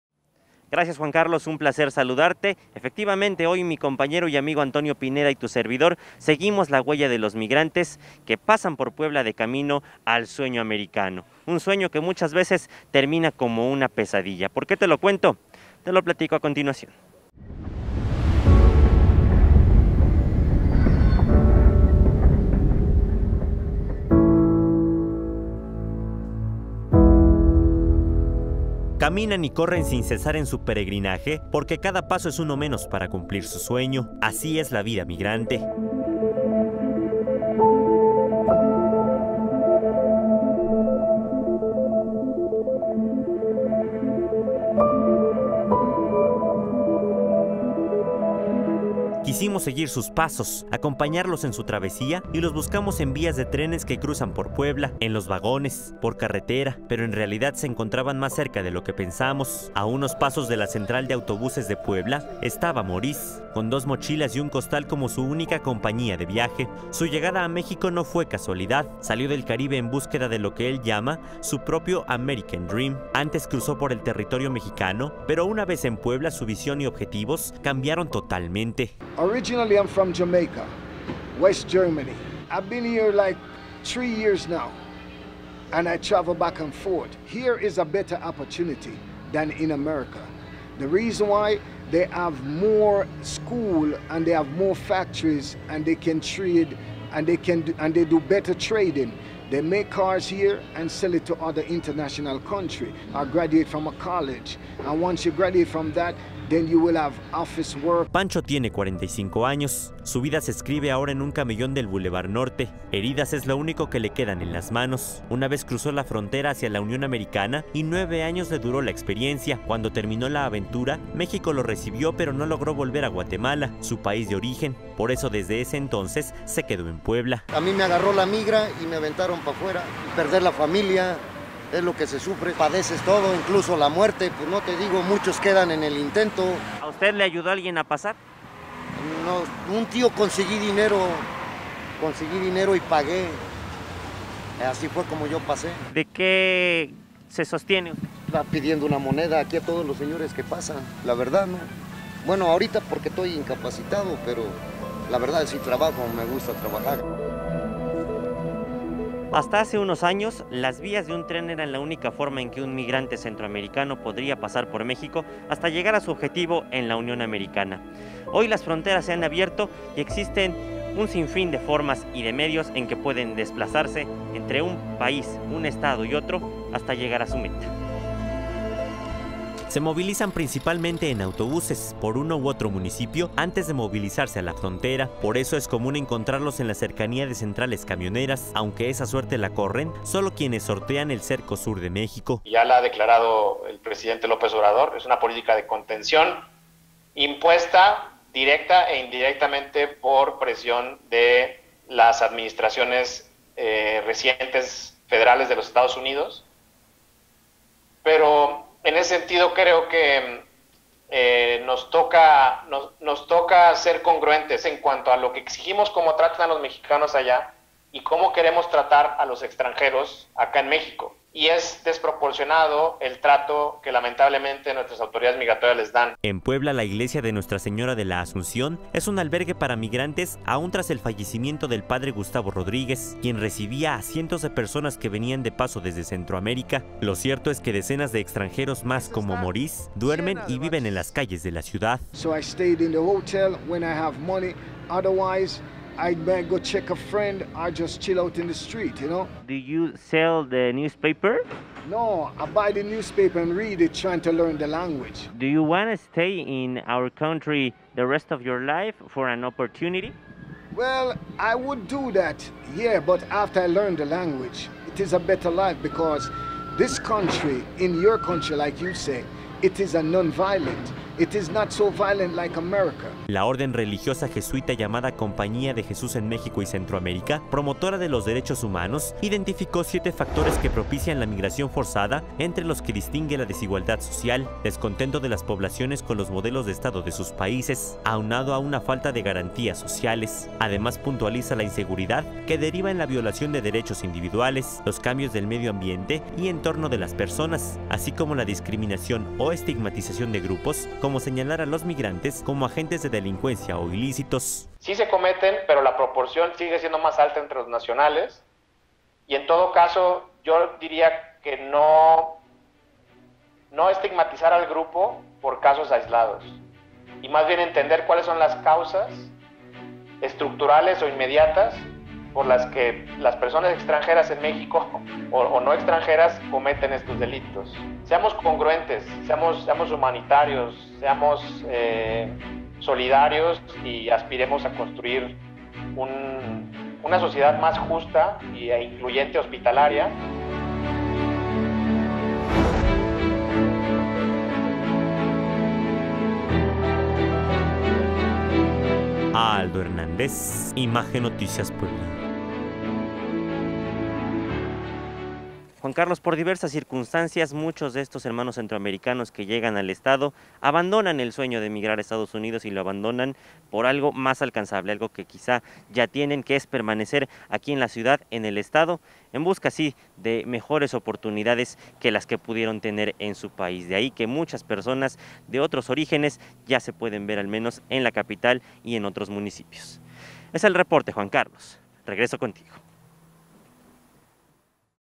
Migrantes y su paso por Puebla || Noticias con Juan Carlos Valerio
Puebla es un punto estratégico para el paso de migrantes que buscan el sueño americano.